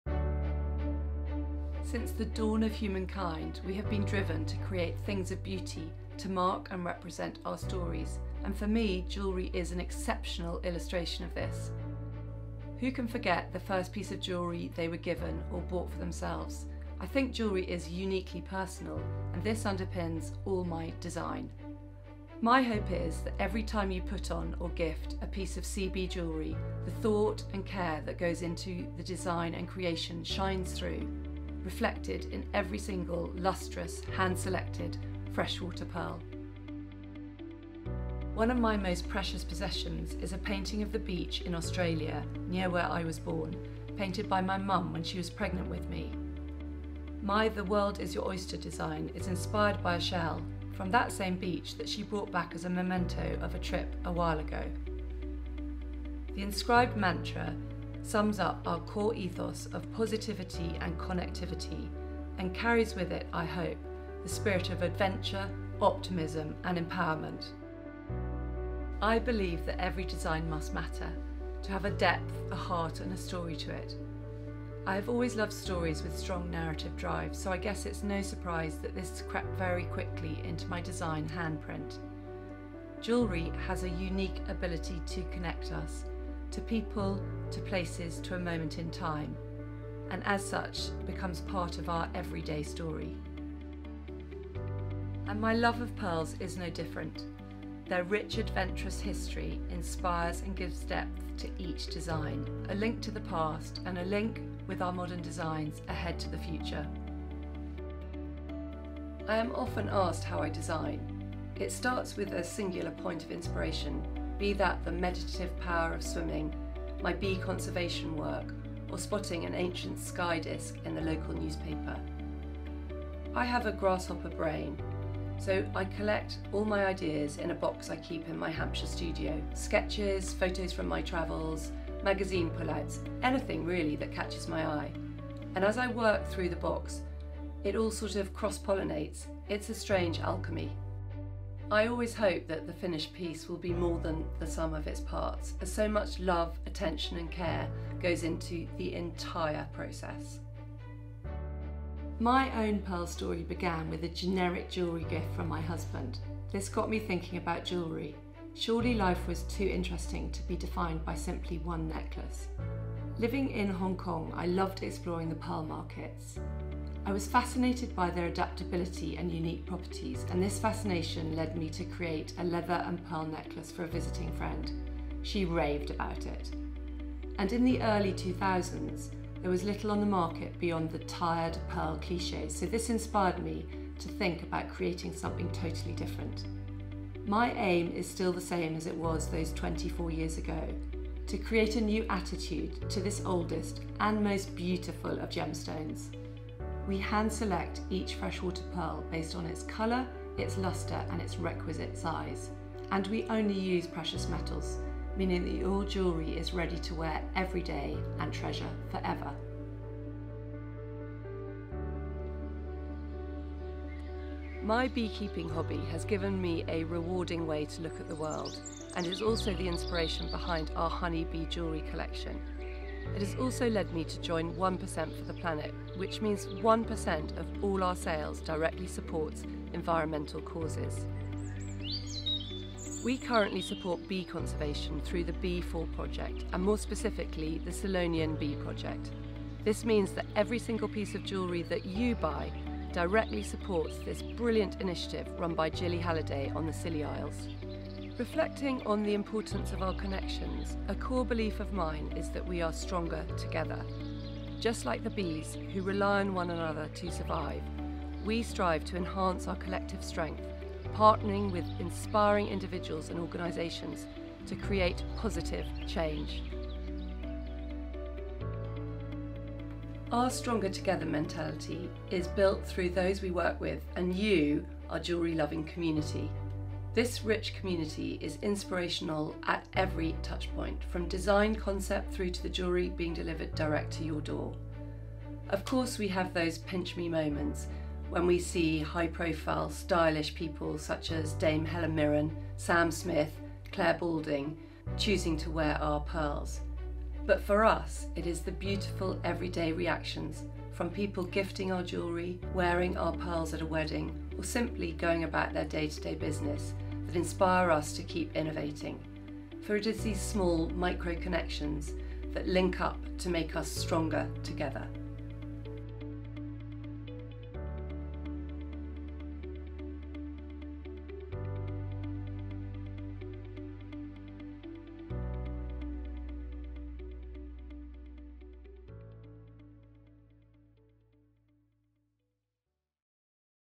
The Claudia Bradby Story: Modern Pearls, Precious Stories
Peek behind the curtain of Claudia Bradby, where every pearl creation tells a story, and embodies a legacy of beauty, craftsmanship and personal connection. From the earliest spark of inspiration to the meticulous design process; Claudia shares the journey of how our pearl jewellery comes to life.

Claudia Bradby: Modern Pearls, Precious Stories.